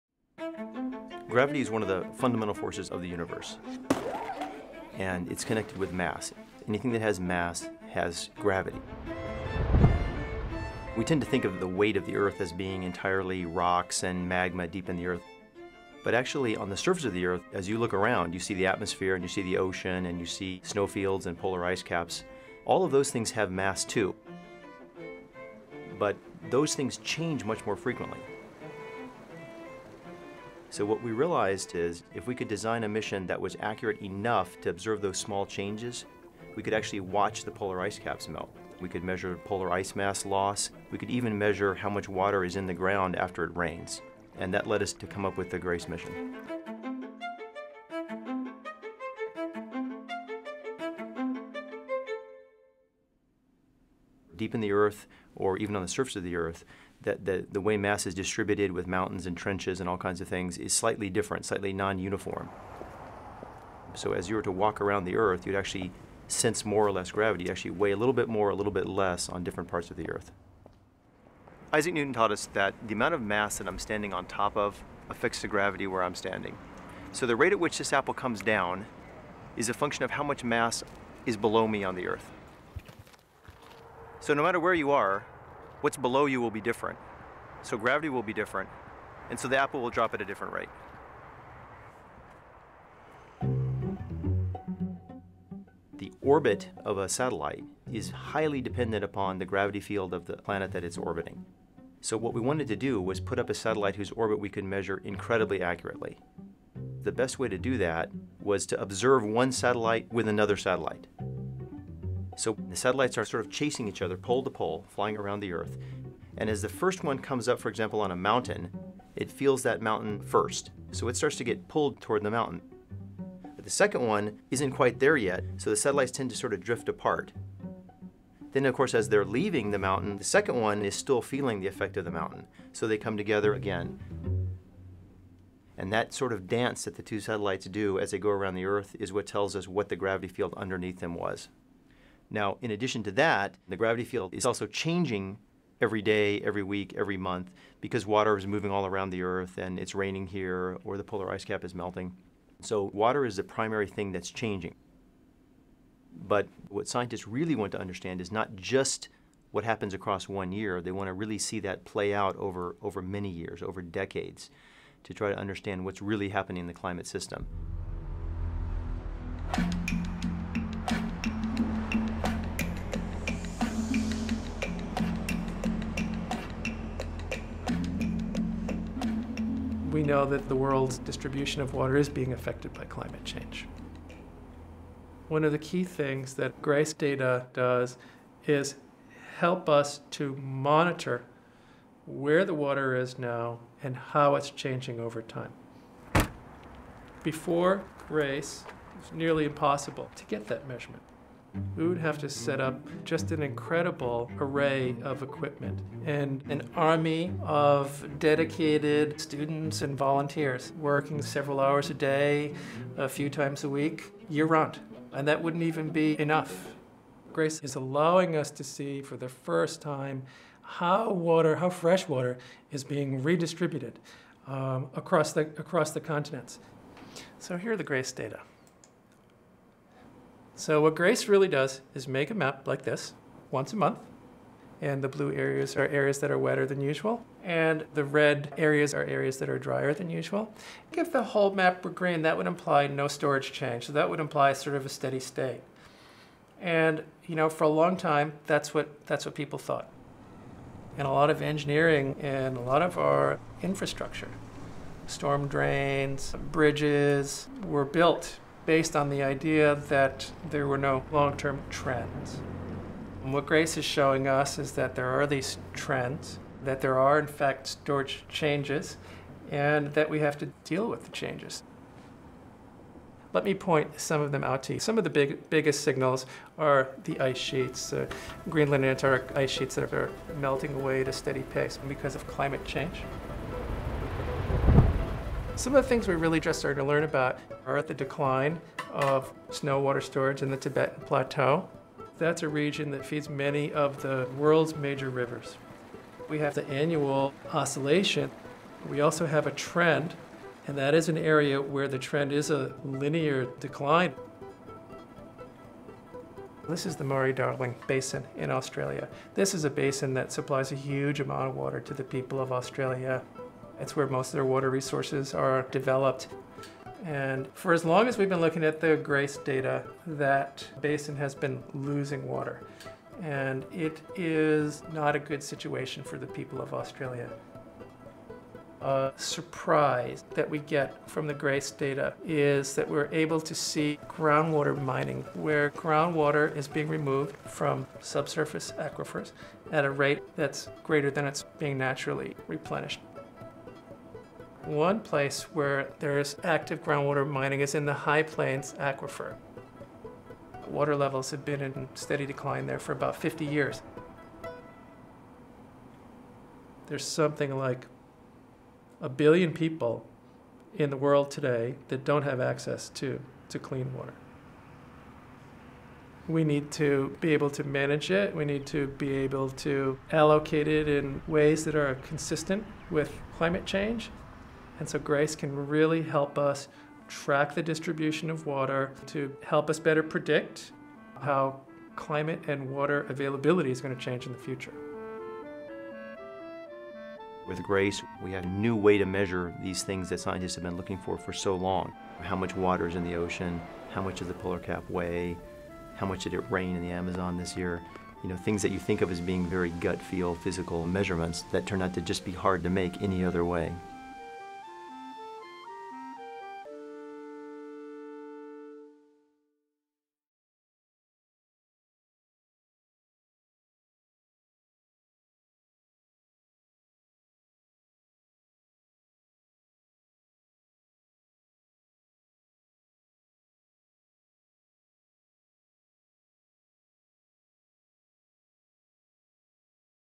NASA GRACE: Tracking water from space
Video credit: American Museum of Natural History

Description of the NASA GRACE program--the first satellites to track the Earth's water movement and distribution. UCCHM's Director, Dr. Jay Famiglietti, describes the Center's work with NASA and what the GRACE satellites are showing us, starting at the 3 minute mark.